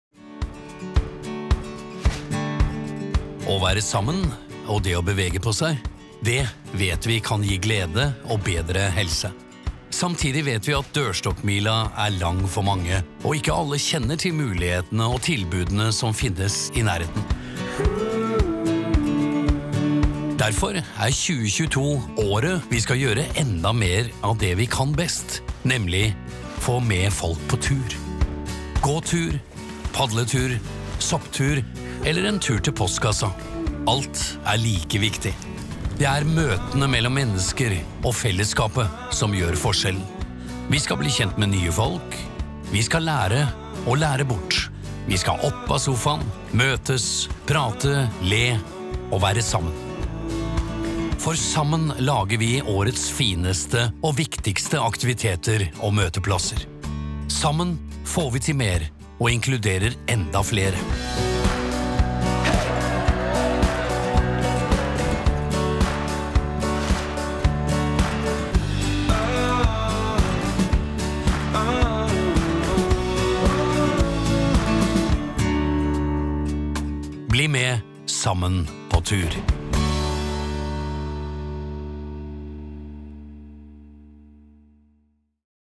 Bli med sammenpåtur i Frivillighetens år 2022!
I 2022 markeres Frivillighetens år. Med signalarrangementet sammenpåtur skal vi lage årets fineste – og viktigste – aktiviteter og møteplasser.

Det er alltid hyggeligere å gjøre ting sammen. Vi vet at det å bevege seg og være ute, spesielt når vi gjør det sammen med andre mennesker, kan gi glede og bedre helse. Samtidig vet vi at dørstokkmila er lang for mange, og at ikke alle kjenner til de mulighetene og tilbudene som finnes i nærheten.

Derfor inviterer vi nå hele Norge til å løfte sammen for å sørge for at enda flere har noen å være ute med. La oss være sammenpåtur!